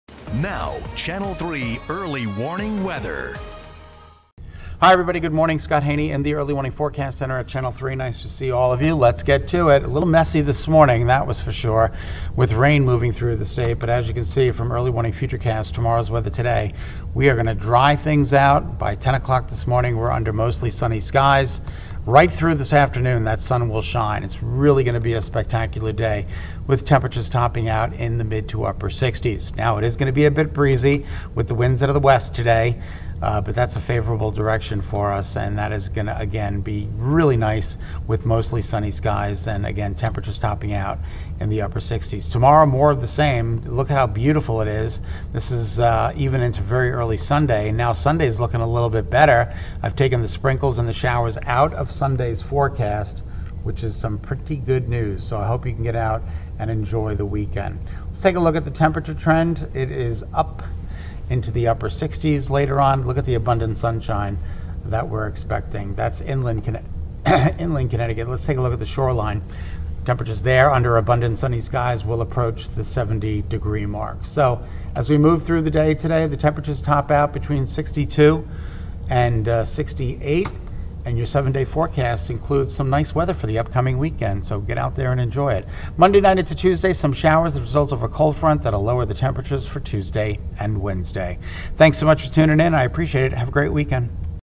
FORECAST: Sun to shine Friday afternoon
Meteorologist Scot Haney said things dry out on Friday, but it will be a bit breezy. Here is his Friday mid-morning forecast.